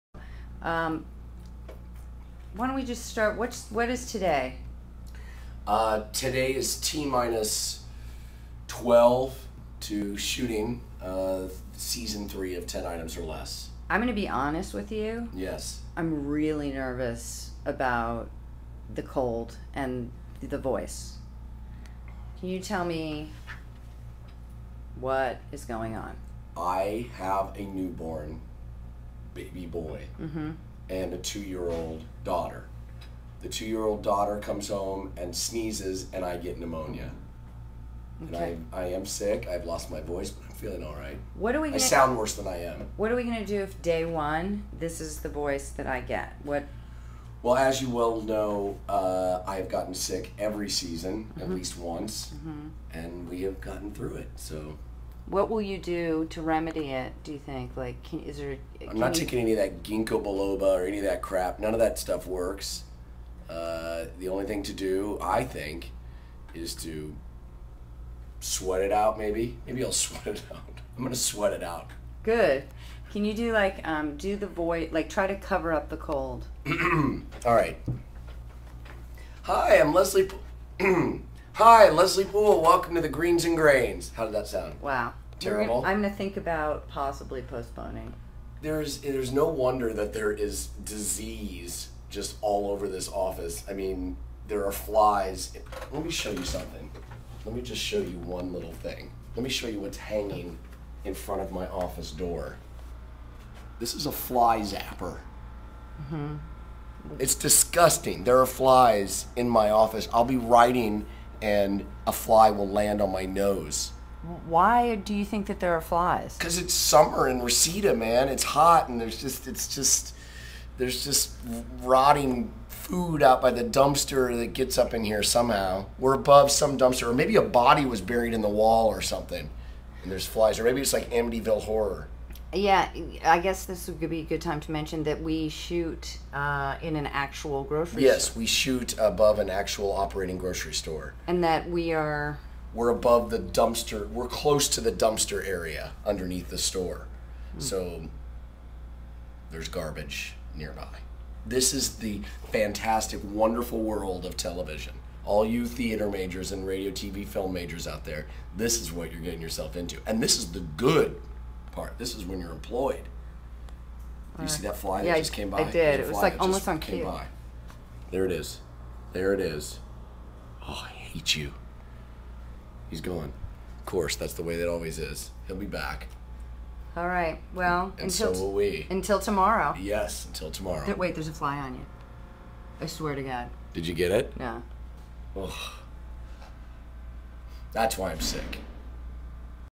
10 Items or Less BTS -- children spread their sickness
We are 12 days from shooting season two of 10 Items or Less but my young kids almost killed me.